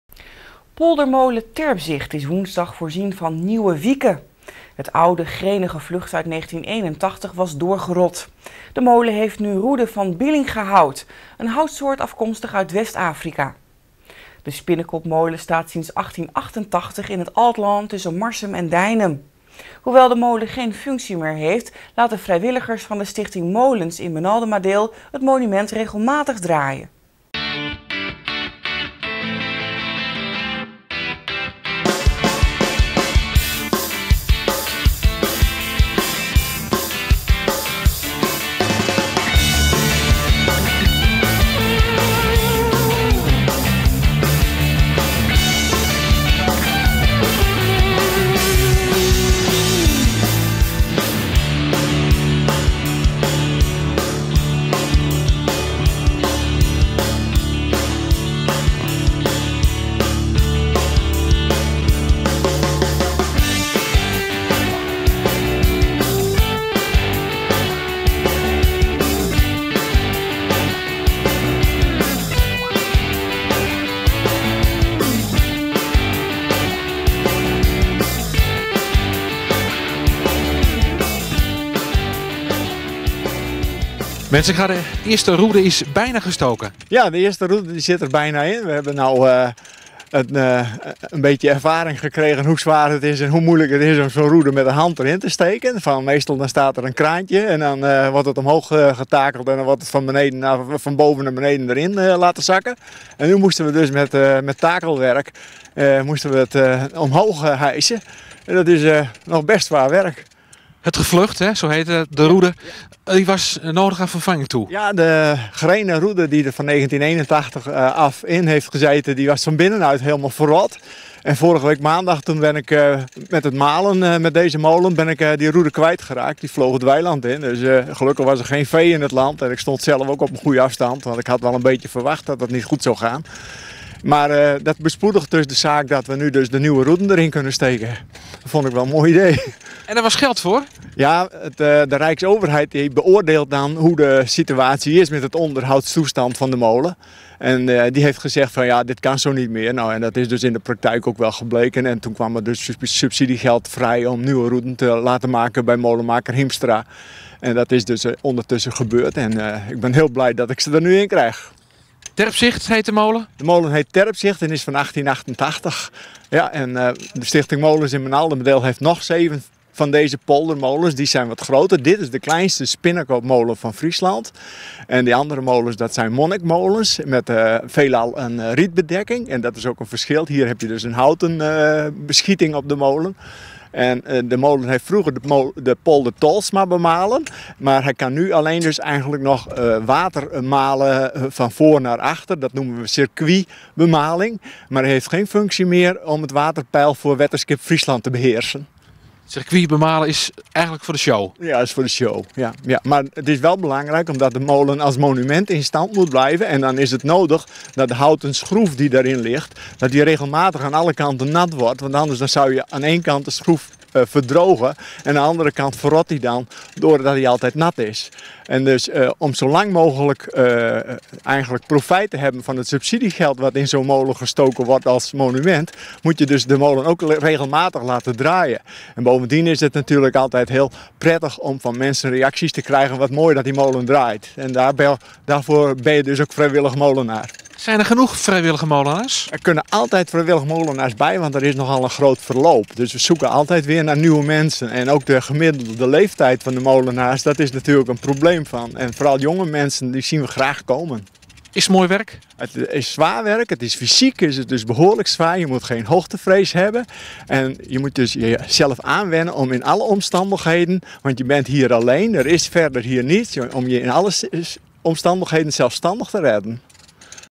GPTV: Poldermolen krijgt nieuwe wieken.mp4
Poldermolen Terpzigt is woensdag voorzien van nieuwe wieken. Het oude grenen gevlucht uit 1981  was doorgerot. De molen heeft nu roeden van Bilinga hout, een houtsoort afkomstig uit West-Afrika. De spinnenkopmolen staat sinds 1888 in het Aldlân tussen Marssum en Deinum. Hoewel de molen geen functie meer heeft, laten vrijwilligers van de Stichting Molens in Menaldumeel het monument regelmatig draaien.